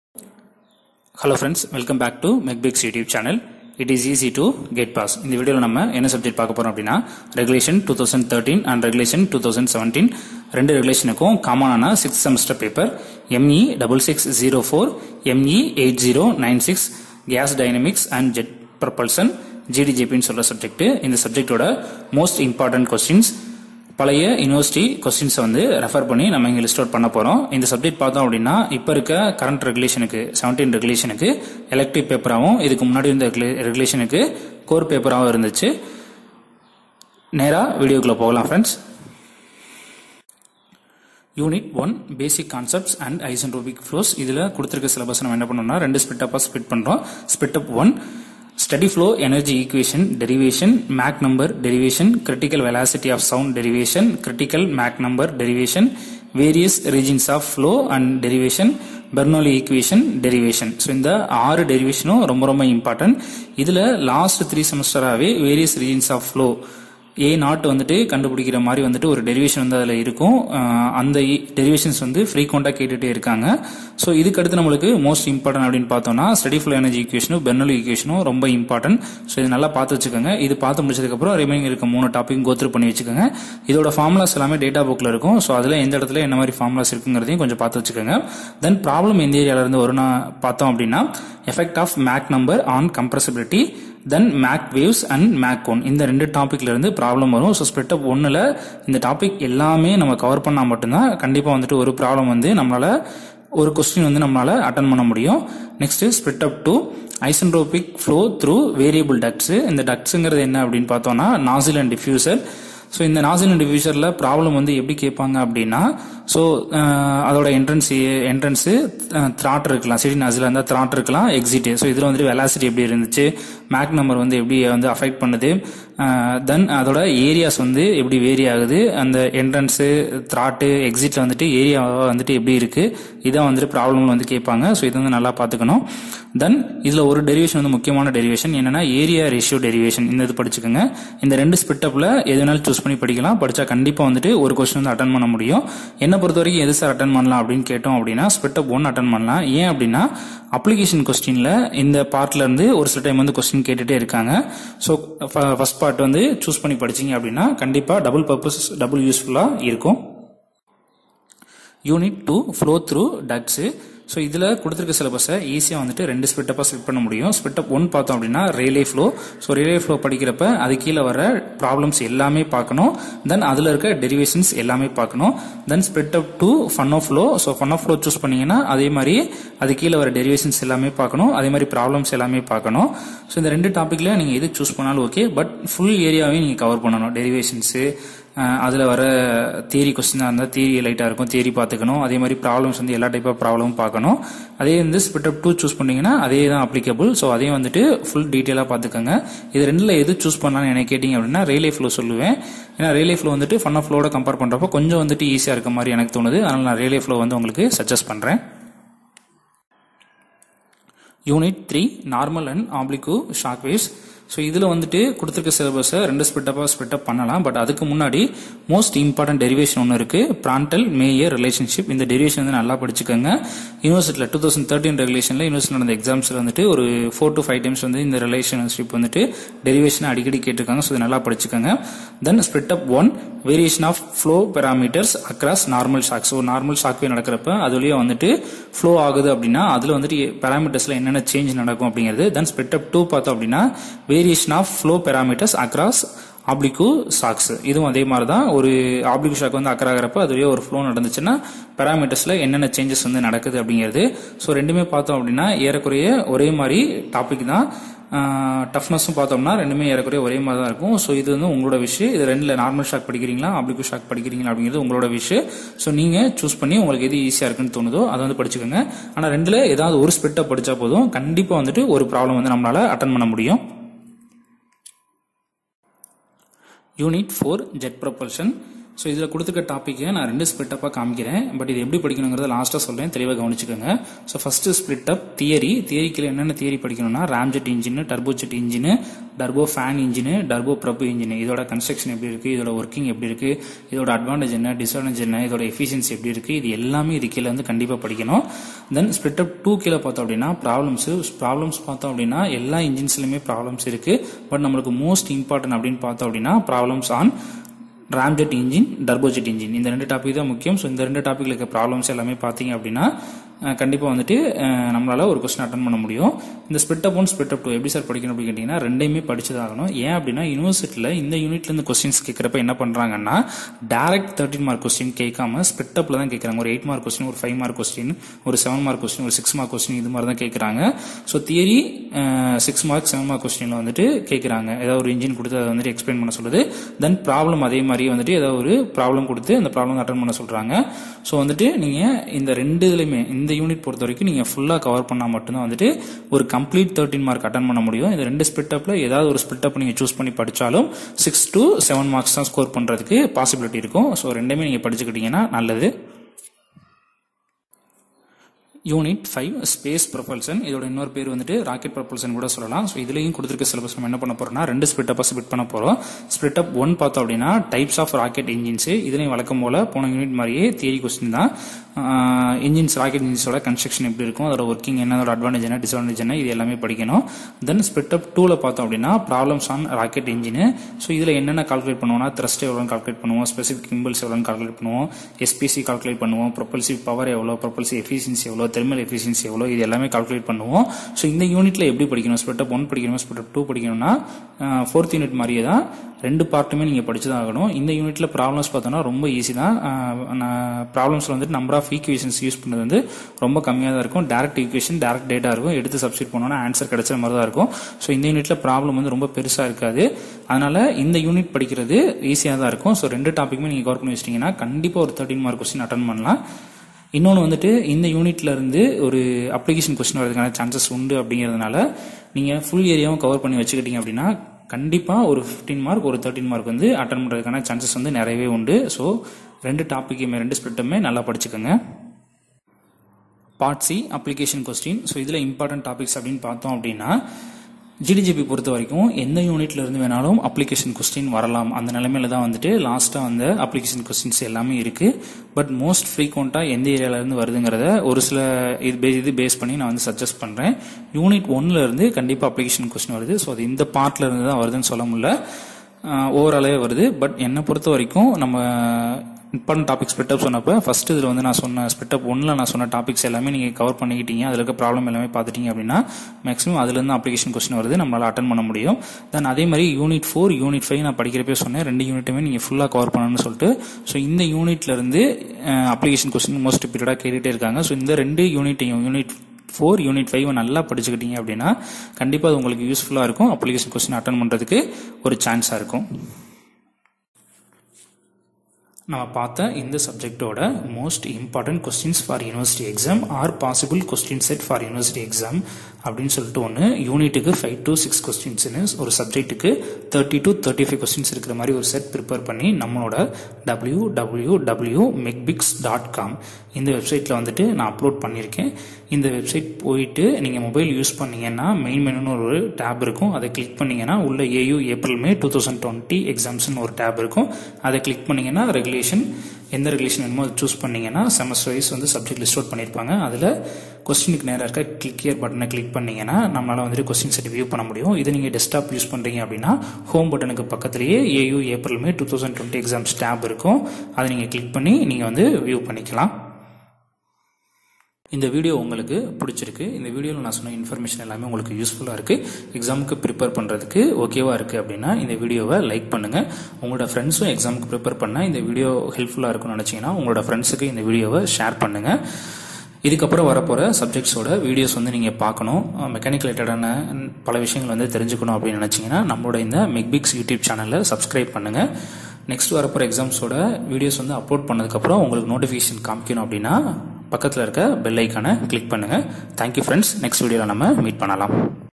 Gas Dynamics and Jet Propulsion Important topics | ME6604 | ME8096 | R2013&17 |GDJP Important topics
This video gives a detailed information about unitwise important topics of " Gas Dynamics and Jet Propulsion" which is so much useful for university examination.This video covers Anna university Regulation 2013 & 2017.

To get possible questions set which may come for university exam, and click AU Apr May 2020 Exams.